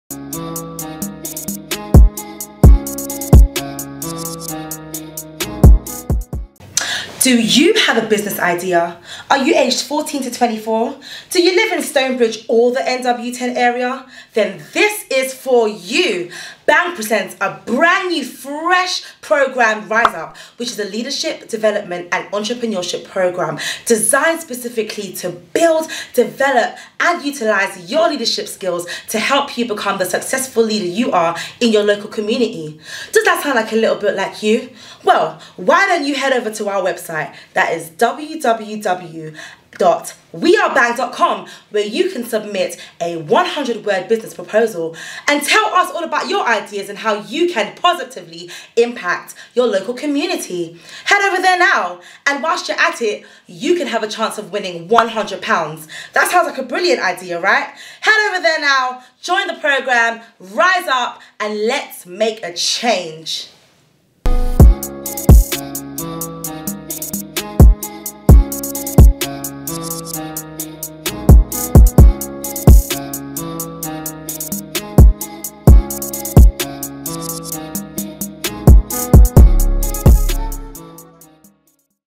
RiseUP - Become A Leader
BANG is excited to launch RiseUP - a project for young people to develop their entrepreneurial and leadership skills.

Develop your business ideas, get a mentor and make a name for yourself in your community.

Young people CAN LEAD!

Young people MUST LEAD!

BANG STANDS FOR YOUNG PEOPLE!

Directed by Derrick Ogole, Digital Marketing Lead